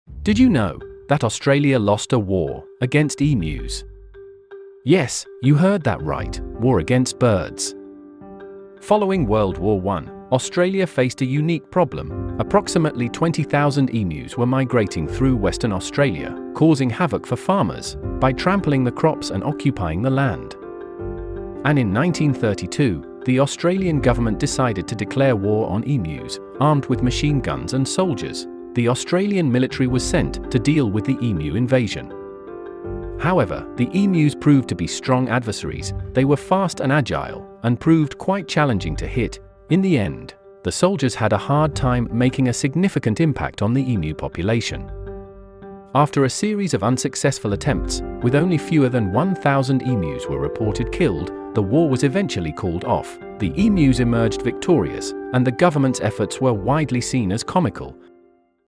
How Australia Lost A War Against Emu!
This is the story of how Australia lost a war against birds